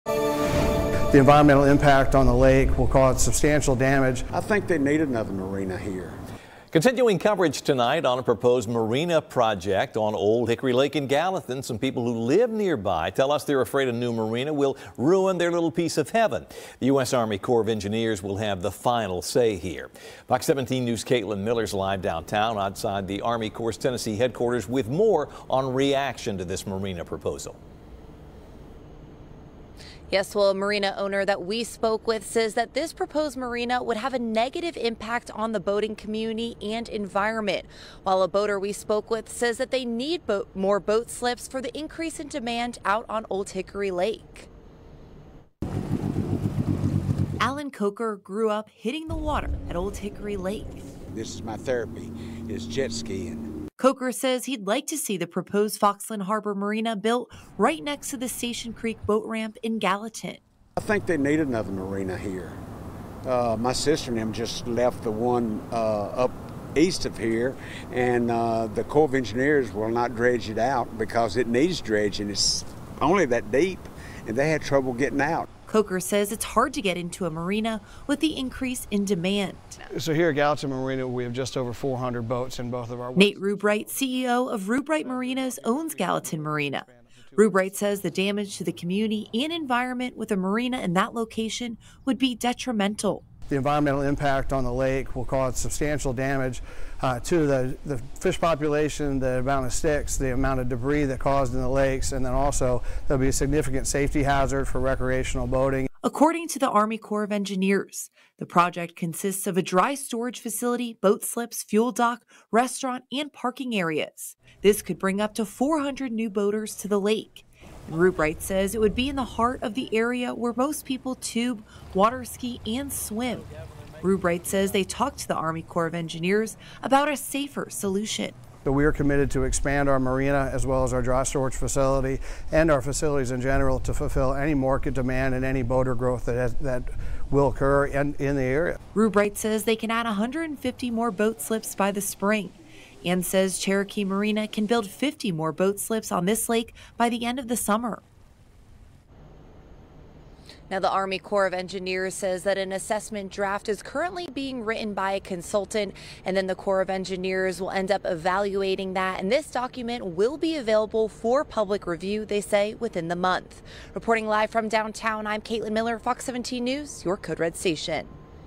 'Significant safety hazard': Proposed private marina continues to spark debate in Gallatin
A proposed marina, Foxland Harbor Marina, on Old Hickory Lake continues to spark controversy.

It is ultimately up to the Army Corps of Engineers to determine if it gets approved.

Allen Coker grew up hitting the water at Old Hickory Lake and he says he'd like to see the proposed Foxland Harbor Marina built right next to the Station Creek Boat Ramp in Gallatin.